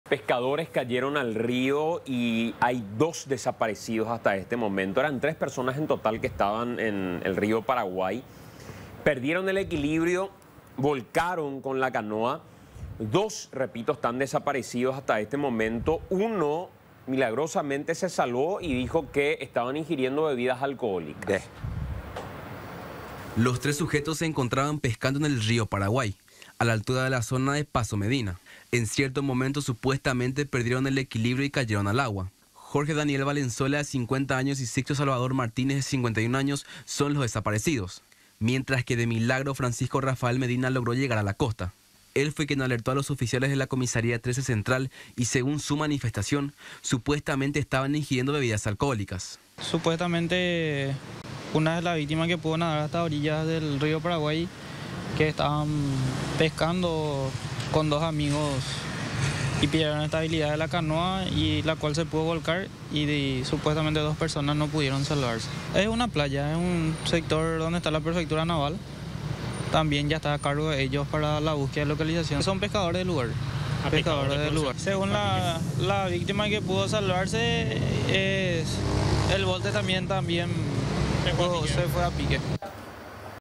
Pescadores cayeron al río Paraguay y dos están desaparecidos
Tres pescadores cayeron al río Paraguay y dos están desaparecidos. El que logró sobrevivir dijo que estaban ingiriendo bebidas alcohólicas cuando volcó la canoa.